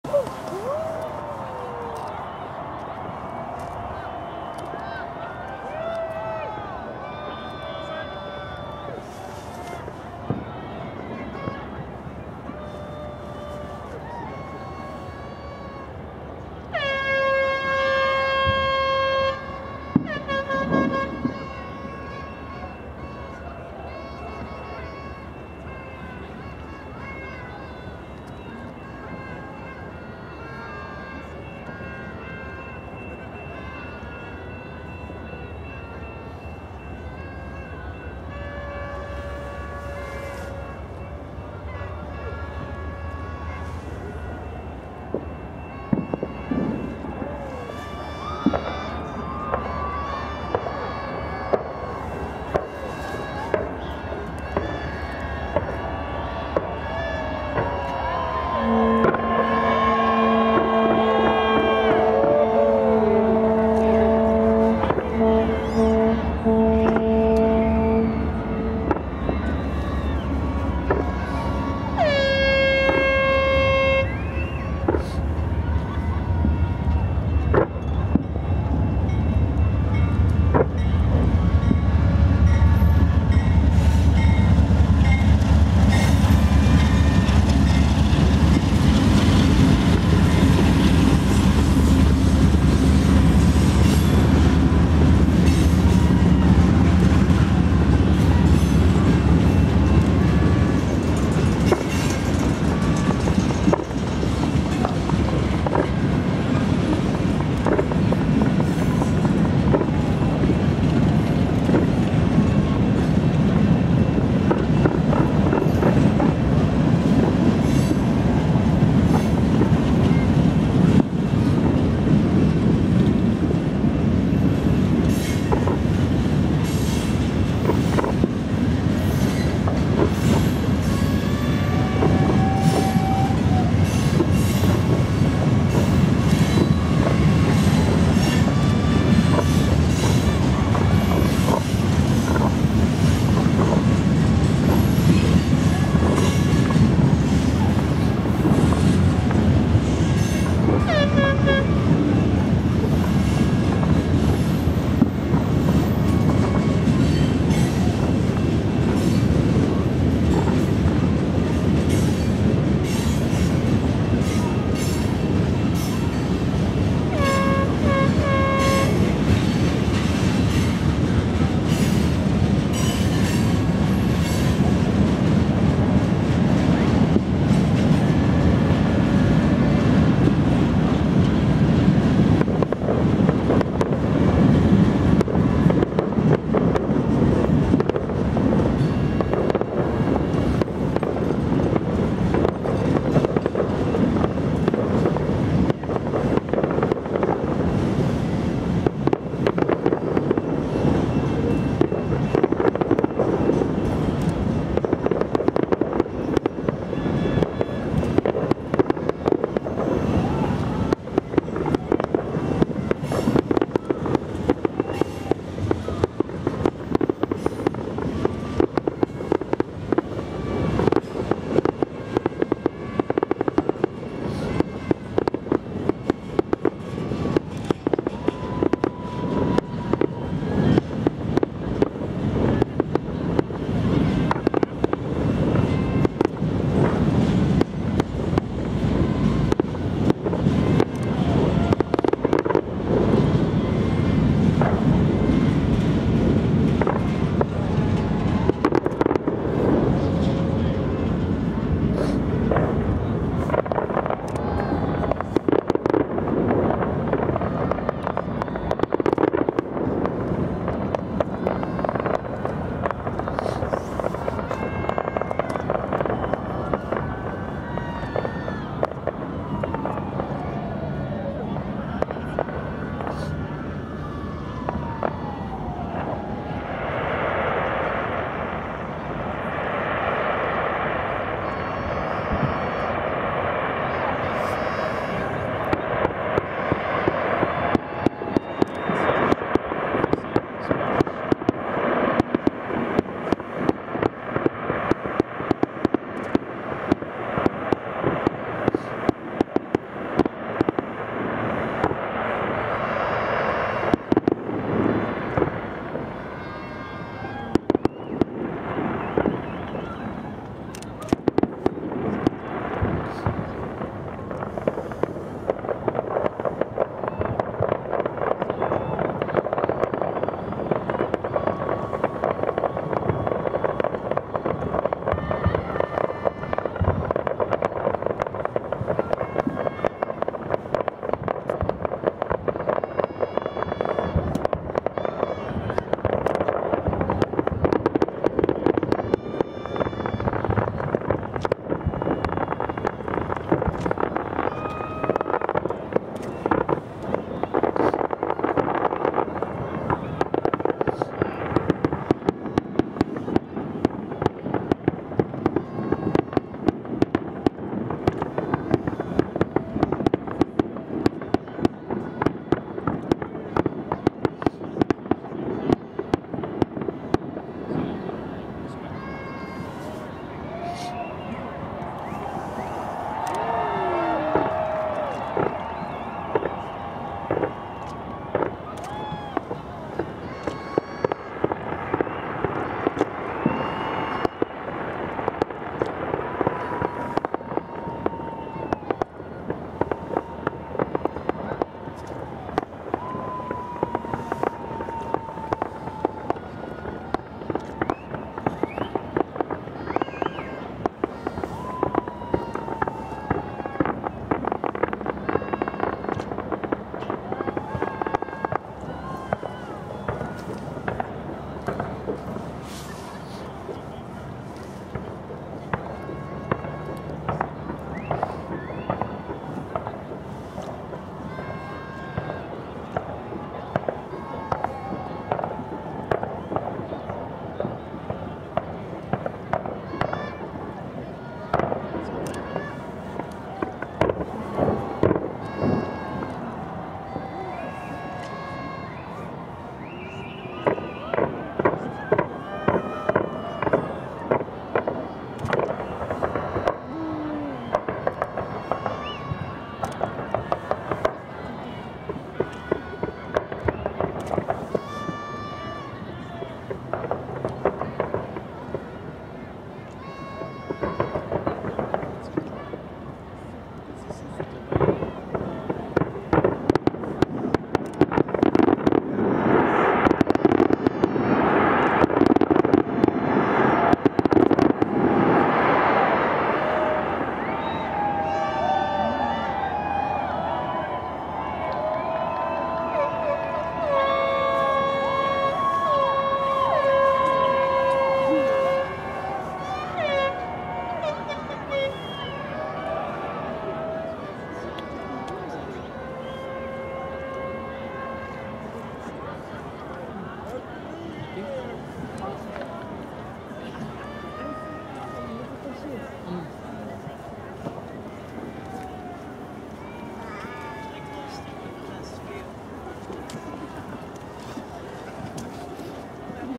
2015 New Year’s Fireworks at the Space Needle
New Years at the Needle

One of the world's largest structure-launched fireworks displays is presented for the 18th consecutive year as the city celebrates the arrival of 2015.